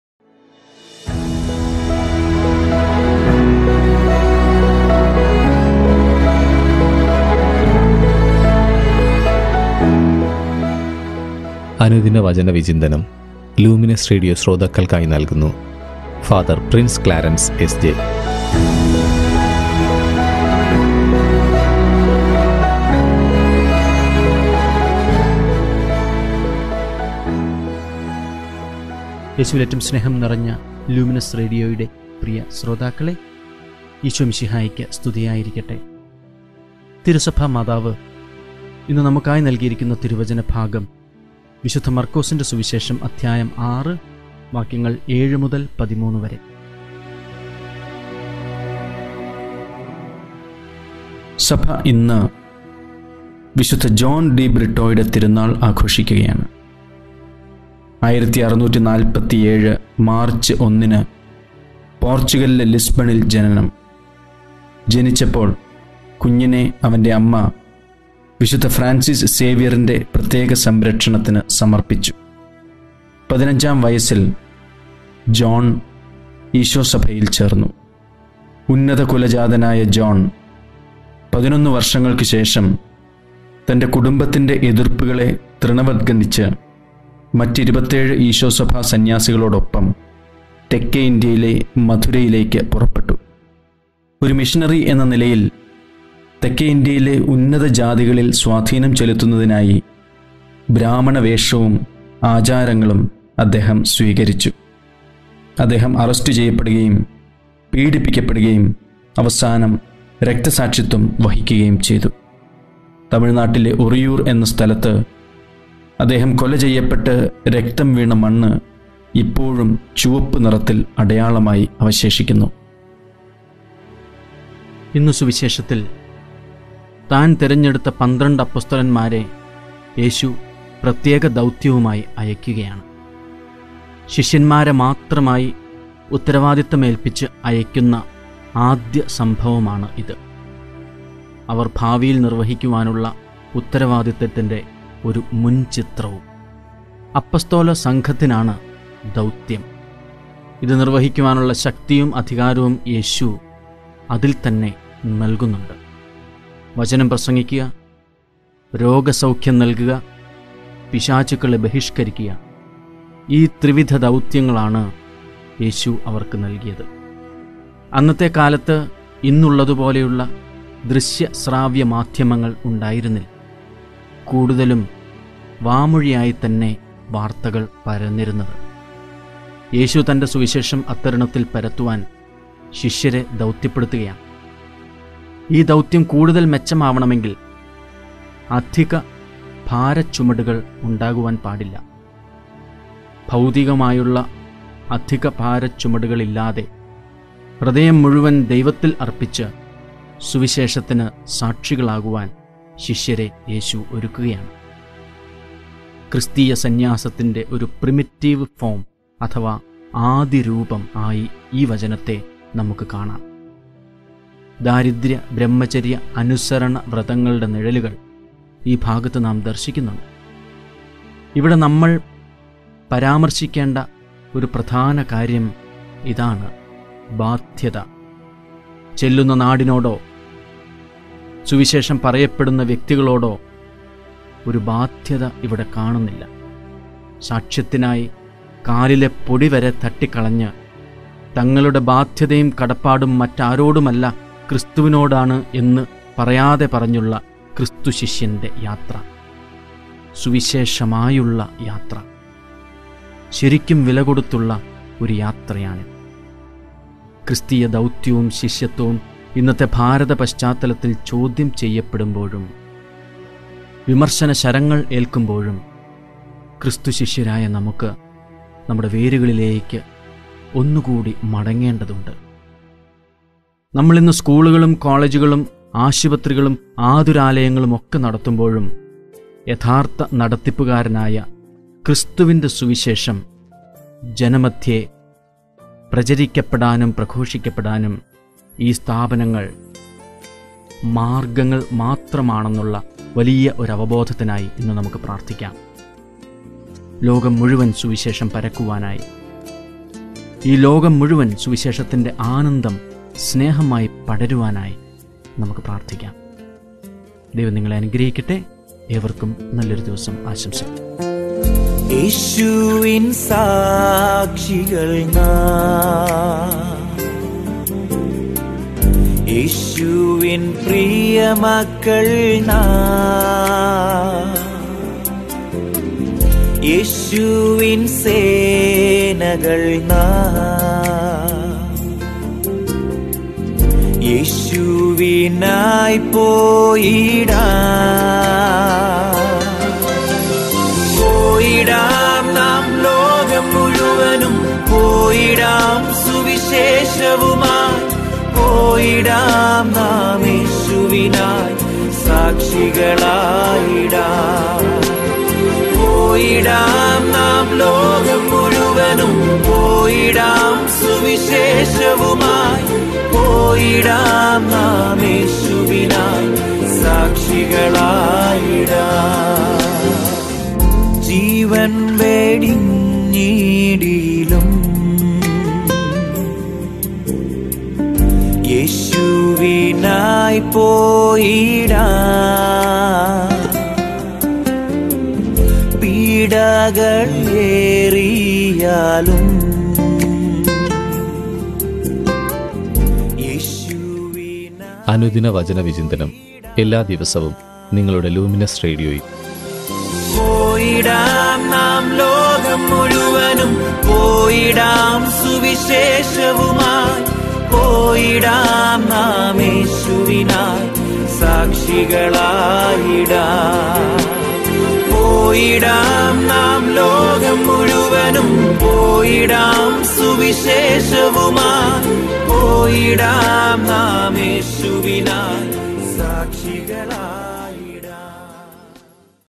On Mission for Jesus | Feb 4 | Daily Gospel Reflection | Malayalam Talk | Fr. Prince Clarence
Daily Gospel Reflection in Malayalam by Fr. Prince Clarence SJ | Malayalam Talk  |  അനുദിന സുവിശേഷം - Feb 04, 2021 -  Mark 6 : 7 - 13

At present Fr. Prince Clarence  S.J. is the director of the Kerala AICUF.

7 He called the twelve and began to send them out two by two, and gave them authority over the unclean spirits.
8 He ordered them to take nothing for their journey except a staff; no bread, no bag, no money in their belts;
9 but to wear sandals and not to put on two tunics.
10 He said to them, “Wherever you enter a house, stay there until you leave the place.
11 If any place will not welcome you and they refuse to hear you, as you leave, shake off the dust that is on your feet as a testimony against them.”
12 So they went out and proclaimed that all should repent.
13 They cast out many demons, and anointed with oil many who were sick and cured them.

Lyrics | Music : Sam Robinson
Vocal : Tom Philip Thomas